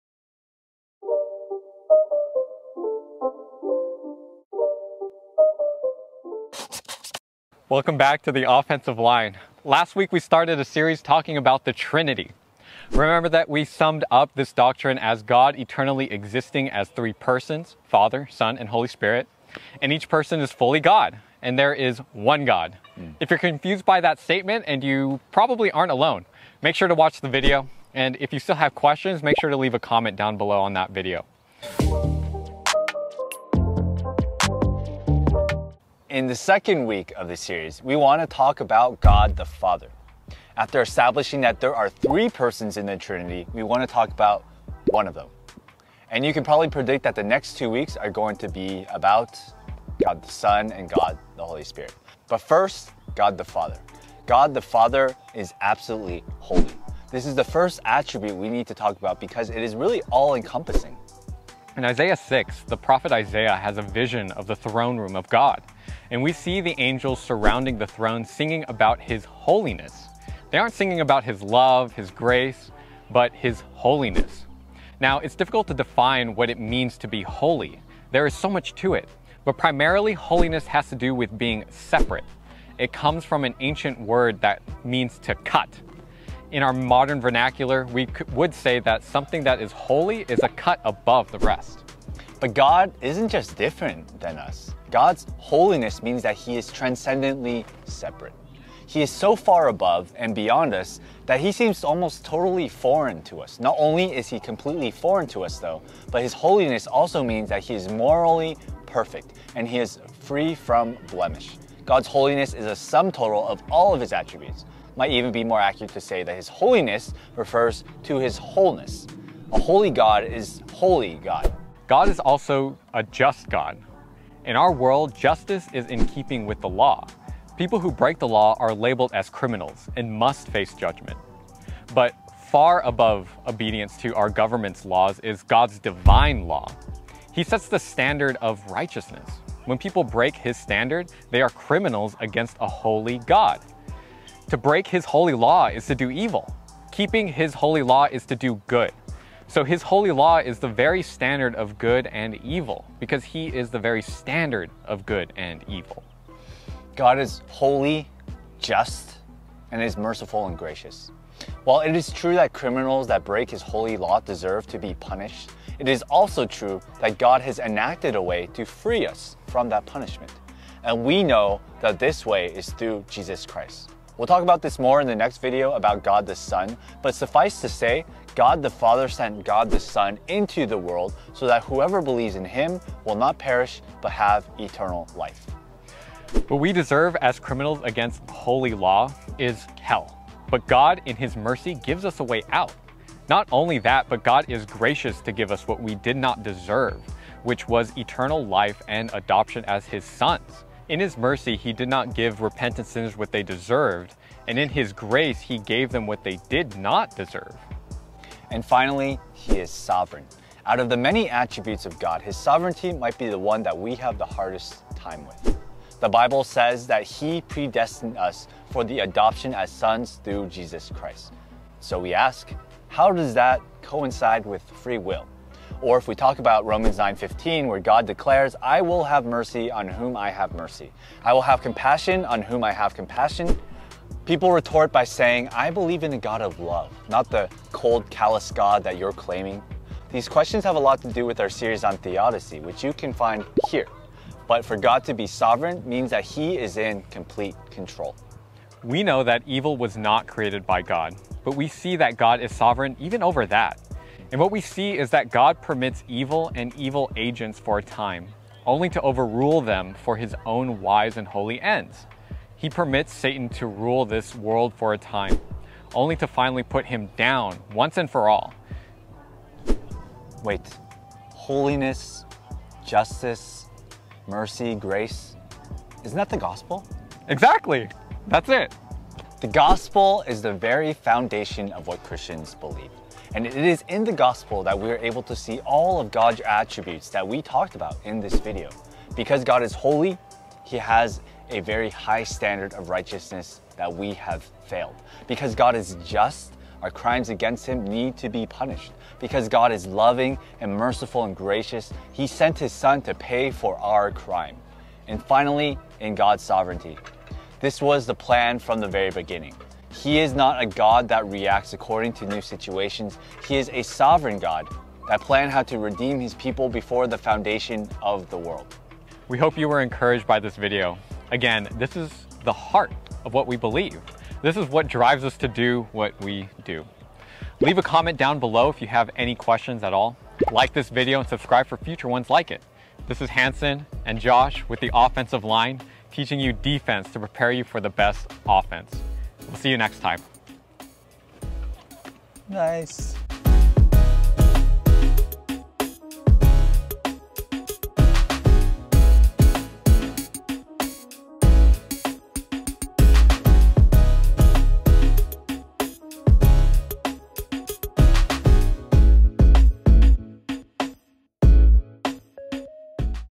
TOL | Who is God the Father?
Who is God the Father? Last week, we started off our mini-series by establishing that there are three persons of the Trinity. This week, we want to dive into each one, beginning with God the Father. As always, let us know if you have any questions!